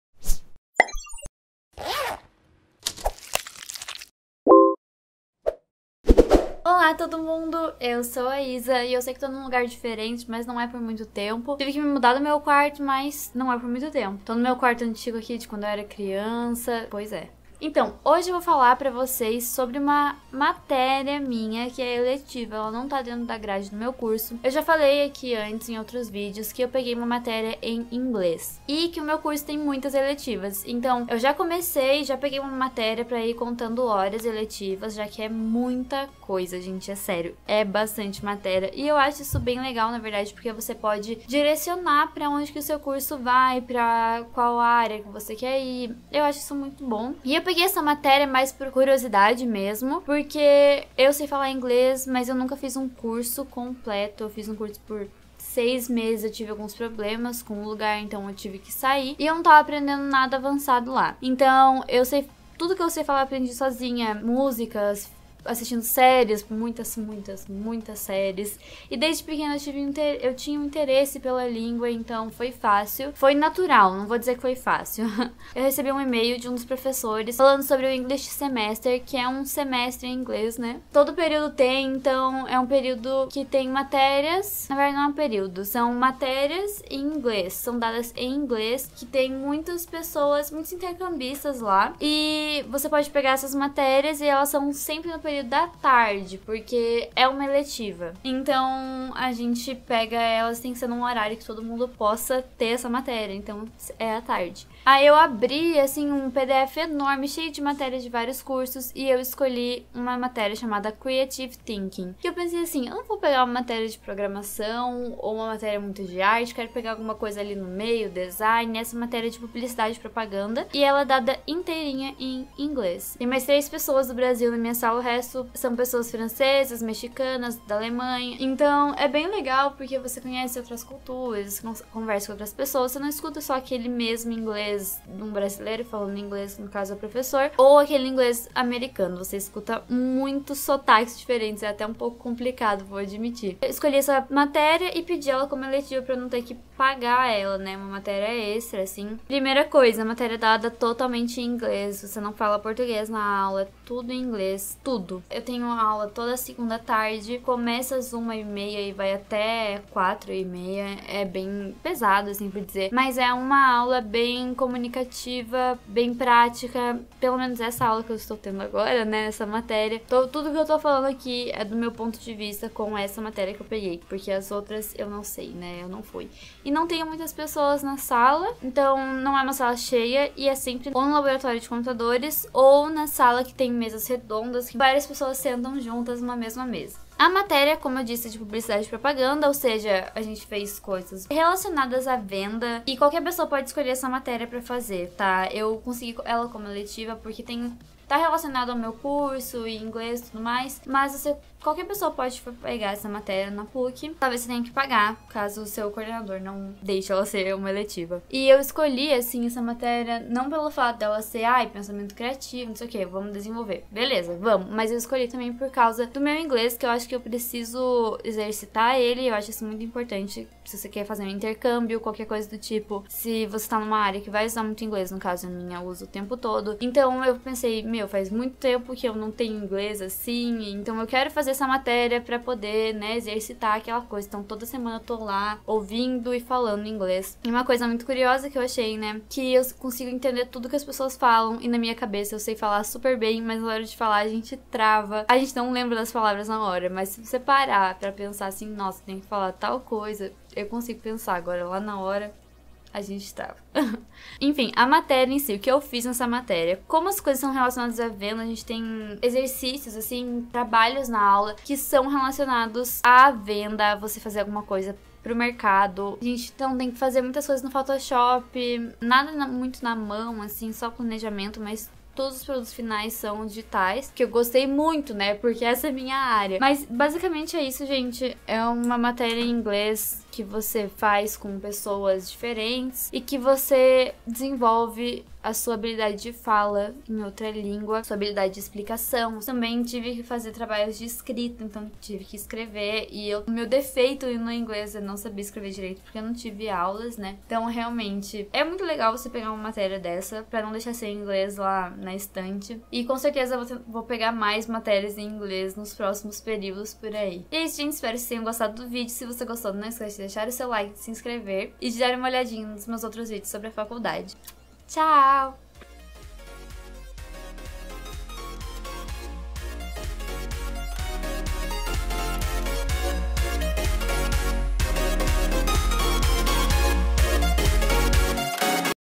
FAÇO UMA MATÉRIA TOTALMENTE EM INGLÊS! Universo Jogos Digitais
Falando sobre uma matéria do English Semester da PUCPR.